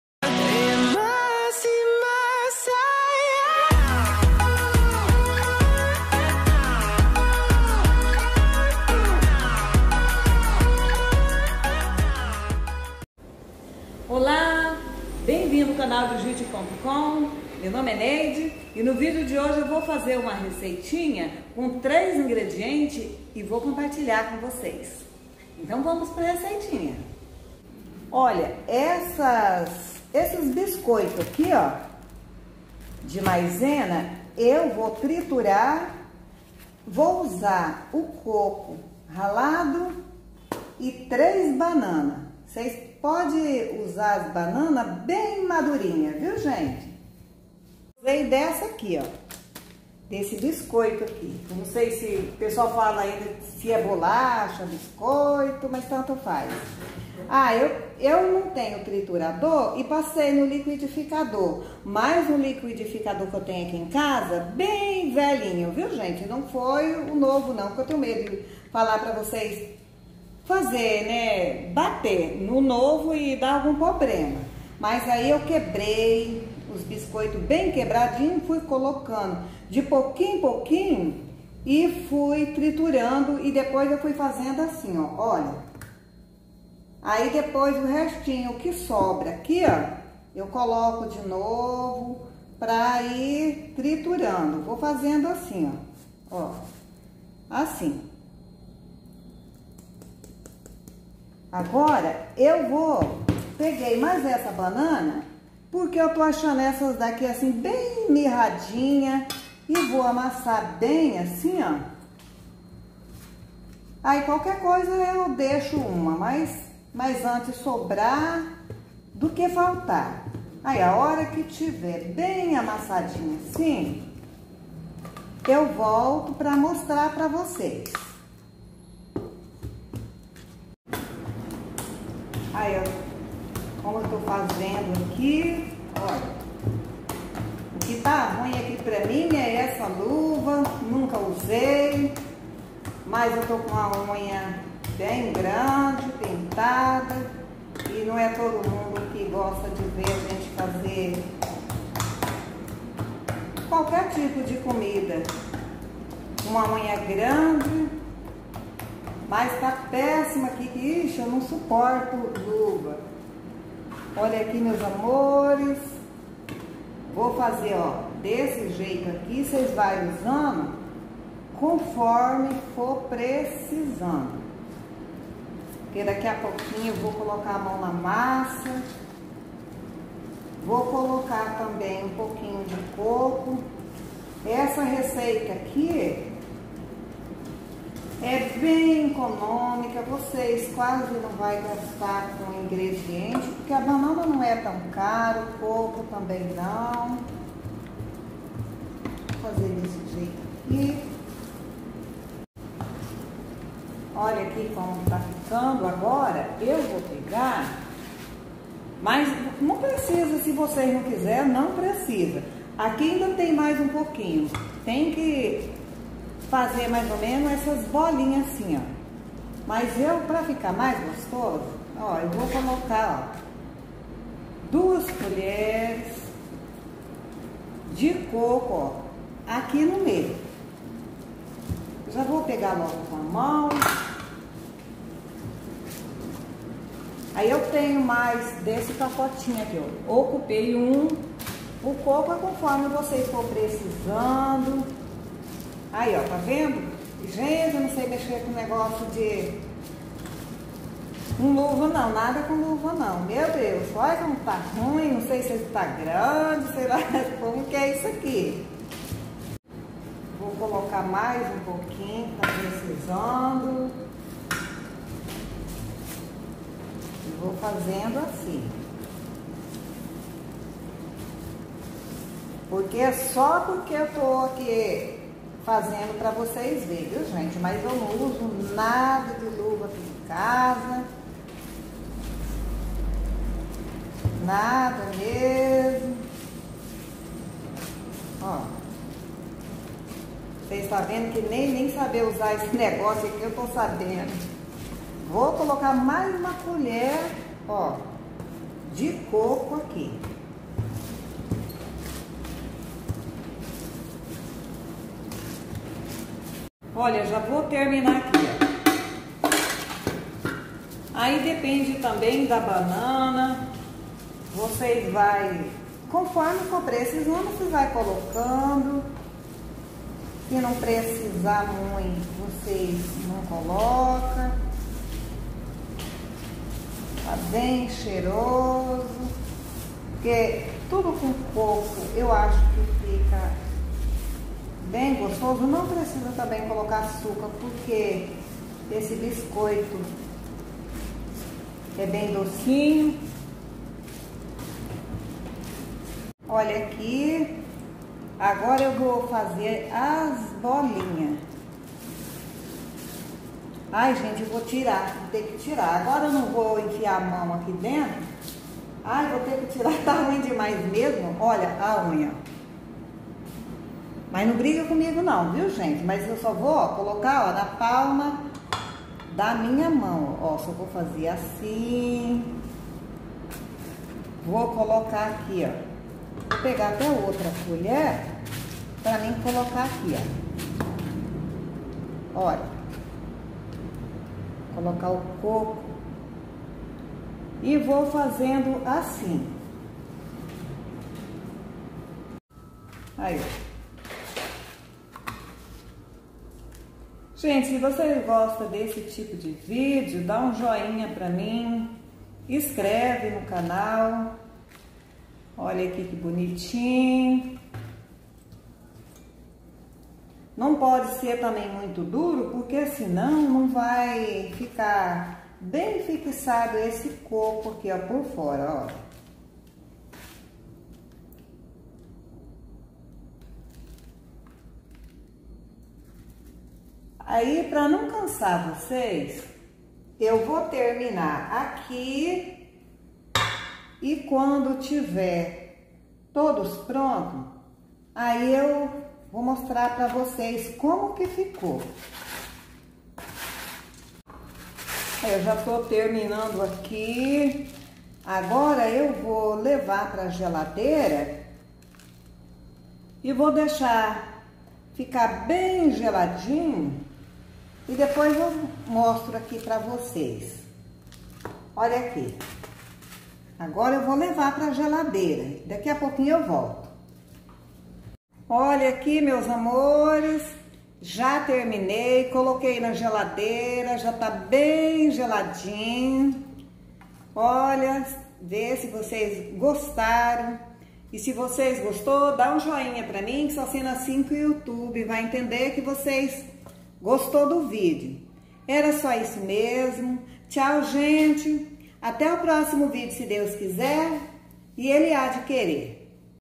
SOBREMESA COM APENAS 3 INGREDIENTES, RÁPIDA E ECONÔMICA
Ingredientes:

1 pacote de biscoito de maisena
1 pacotinho de coco ralado
4 bananas nanicas maduras